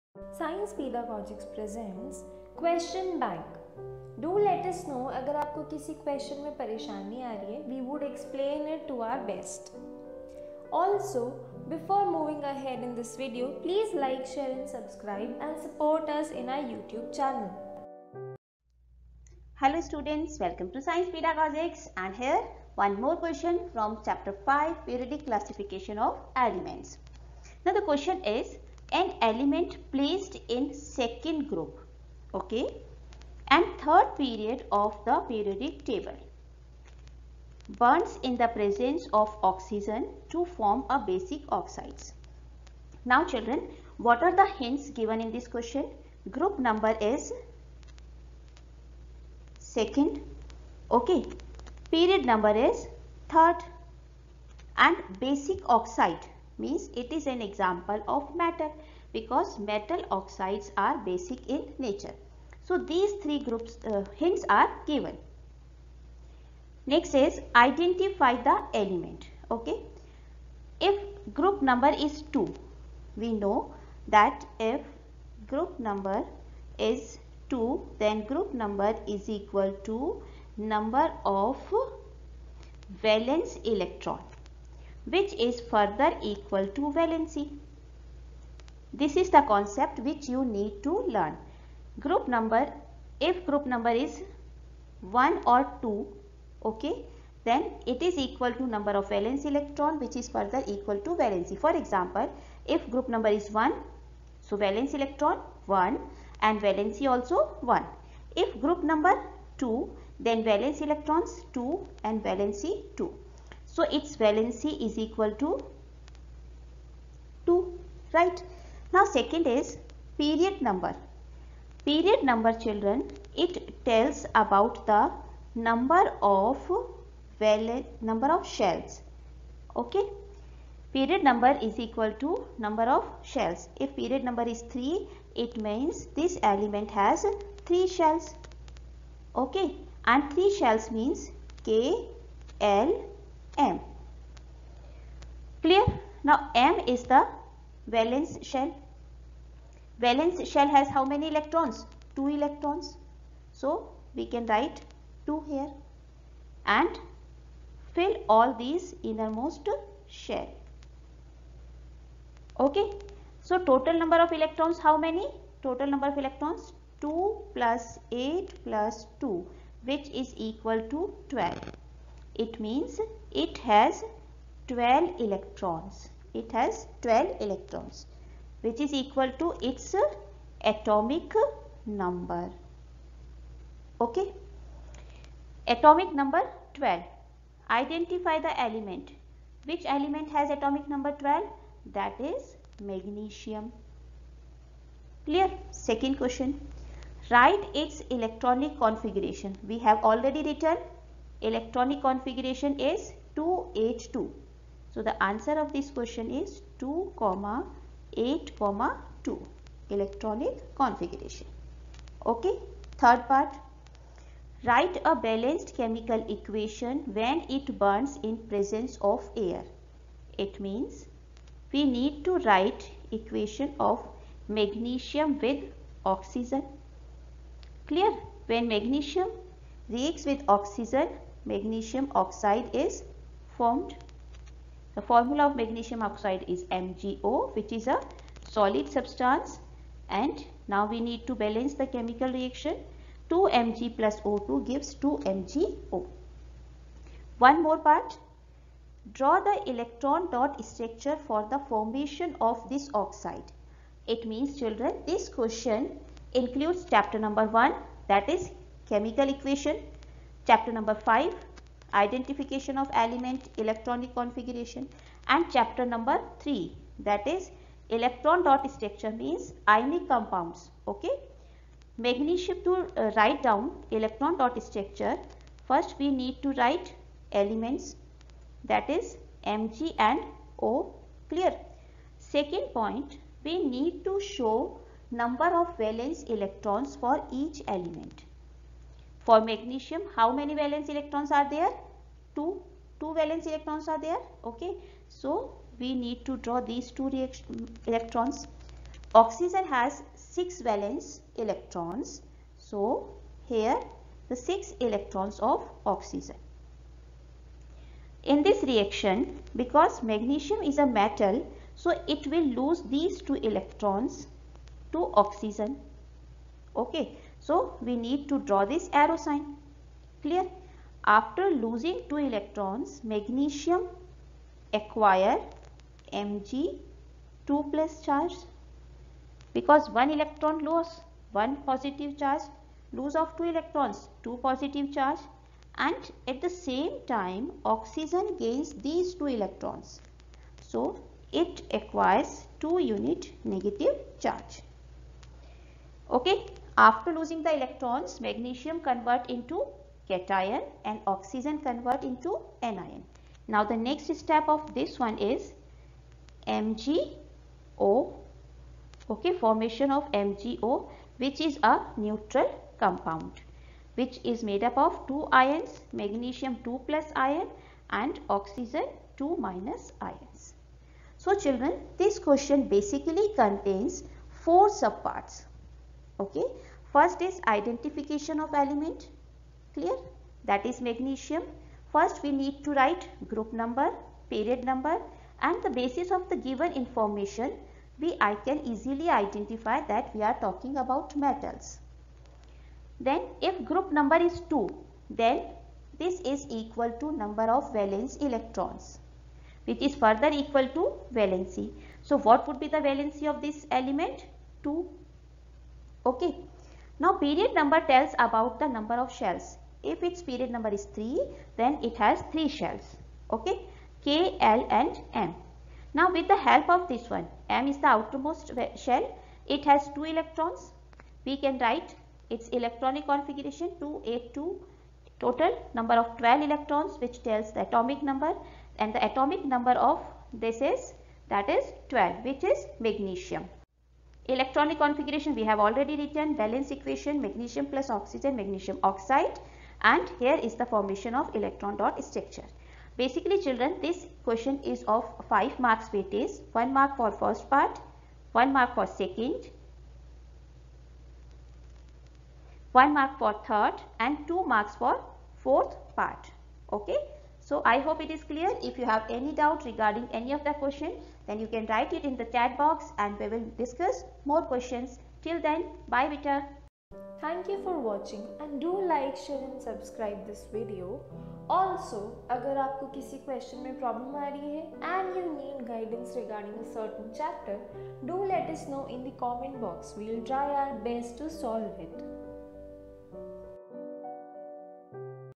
Question Bank | Class X | Chemistry | Science
This is a series of previous year board examination questions of class 10 science. Here answers are explained in simple language with weightage of marks also. We hope that these will helpful for students.